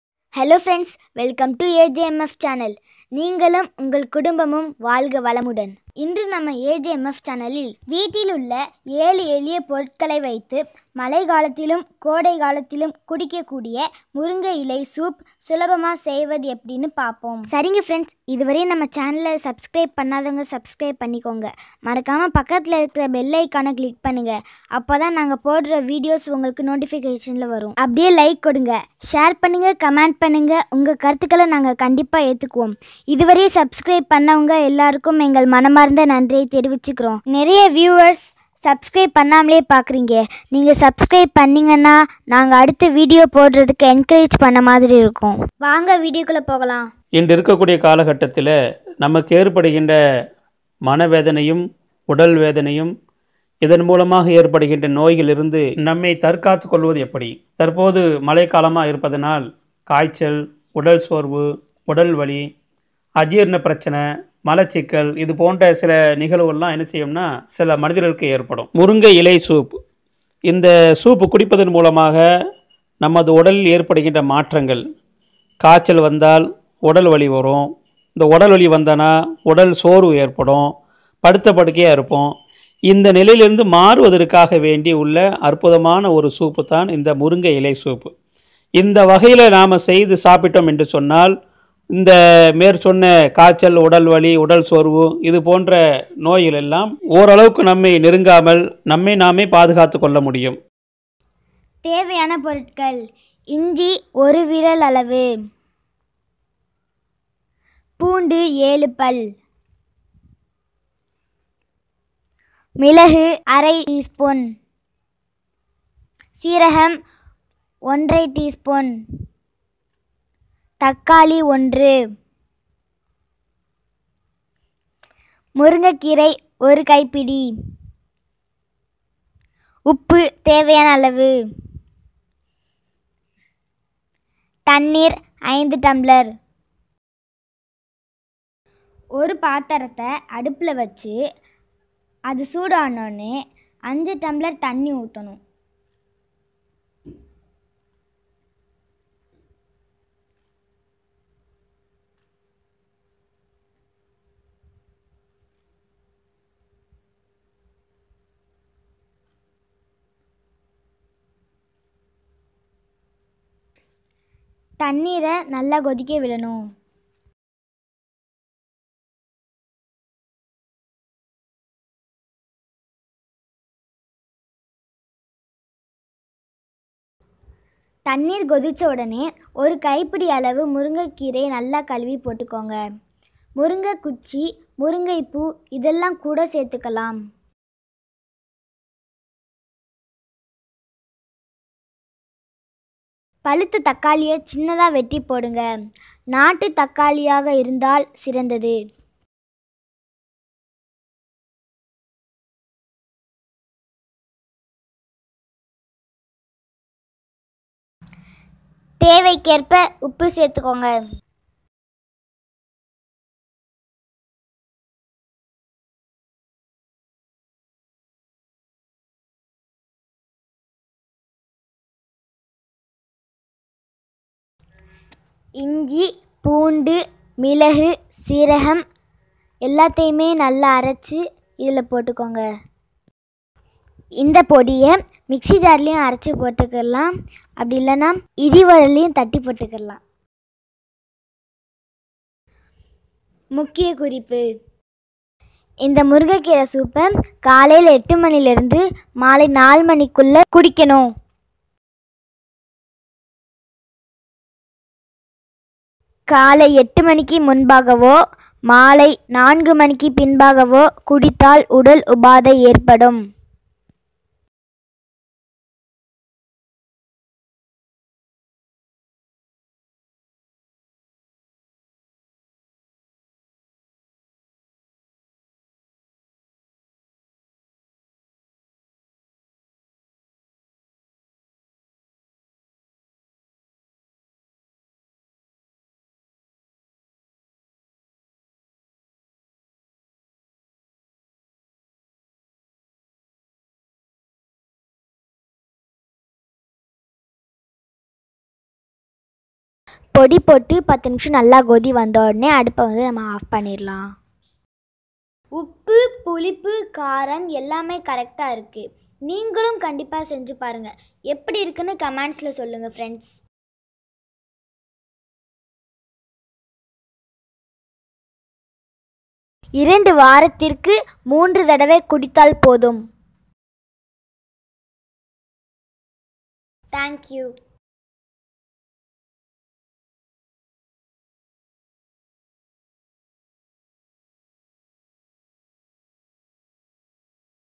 முருங்கை கீரை சூப் செய்வது எப்படி?? குடிக்கும் நேரம், அதன் பயன்கள்...
Guys, in this video : முருங்கை கீரை சூப் குடிக்கும் நேரம்,  சூப் செய்வது எப்படி, அதன் பயன்கள்... Drumstick leaves soup recipe in Tamil...

Murungai Keerai Soup is a healthy, tasty and make it easily.. Drumstick soup builds immunity power against joint pain, Gas problem, Gas disturbance, fever, eye diseases and hair growth.

INGREDIENTS :-
1. Ginger - 1 finger size
2. Garlic - 7
3. Pepper - 1/2 tspn
4. Cumin seeds - 1 1/2 tspn
5. Tomato - 1
6. Drumstick leaves - 1 handle size
7. Salt - as per need
8. Water - 5 glass

( ginger, garlic, pepper, cumin seeds - crushed )

Details explain in Tamil Language.

This information contained in it is useful to you..

AJMF CHANNEL